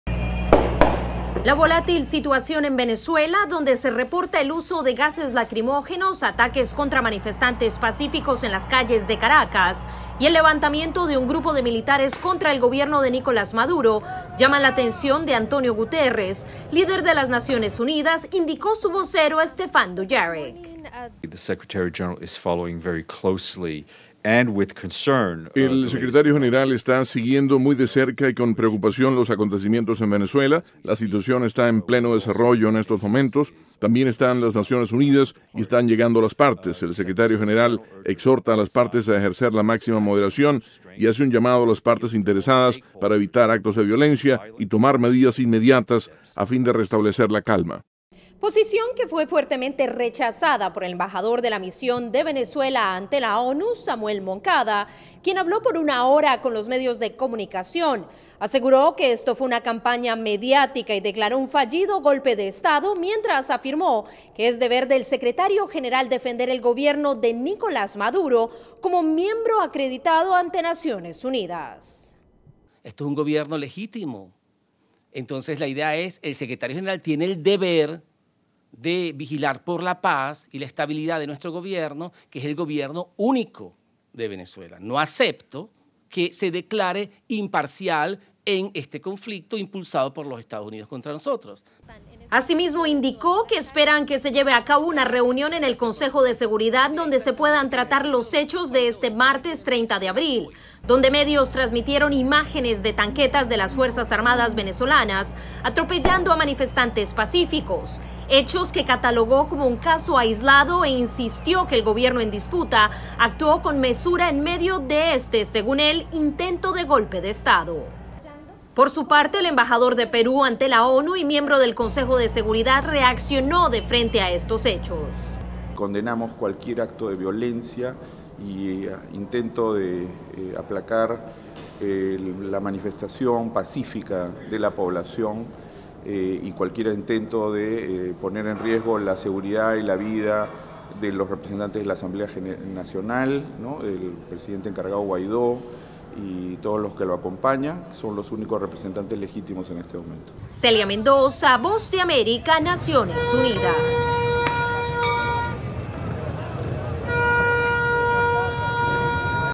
ONU llama a la calma en Venezuela
Naciones Unidas sigue de cerca los hechos que se registran la capital de Venezuela. El Secretario General del organismo hizo un llamado a la calma y manifestó que han contactado a las partes involucradas.
Publicado originalmente